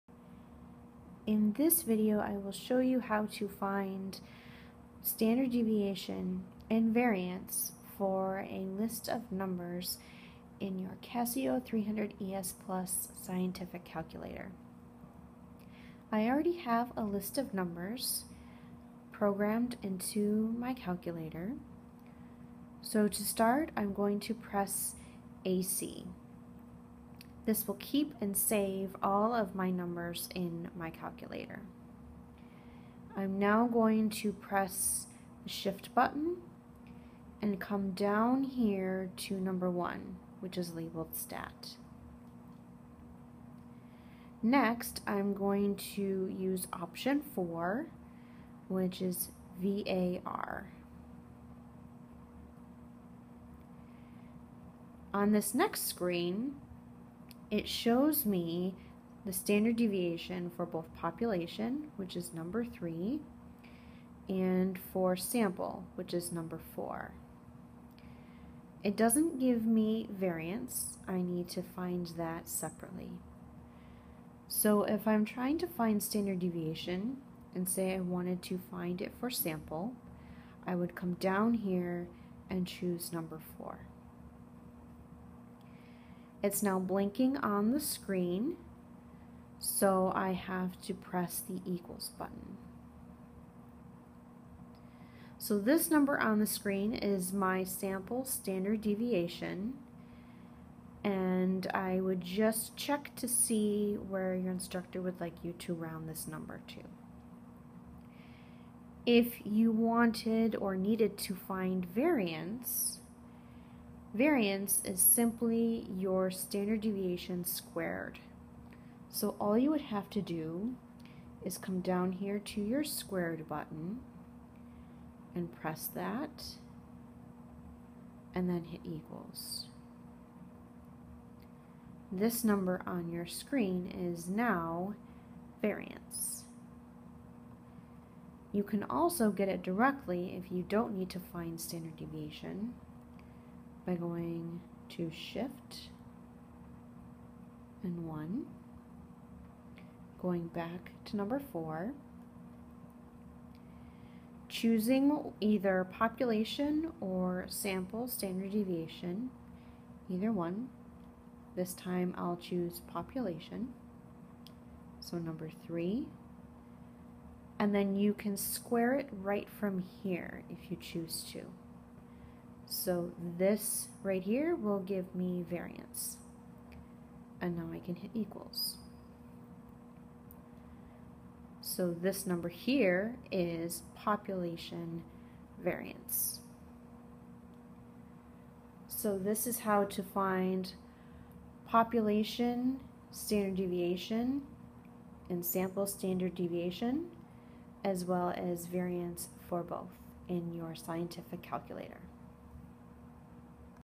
Finding Standard Deviation and Variance for Casio 300ES Plus Scientific Calculator
This video will show how to find standard deviation and variance for one variable statistics in Casio 300ES Plus Scientific Calculator.  This process is similar for those who own a Casio 115ES Plus Scientific Calculator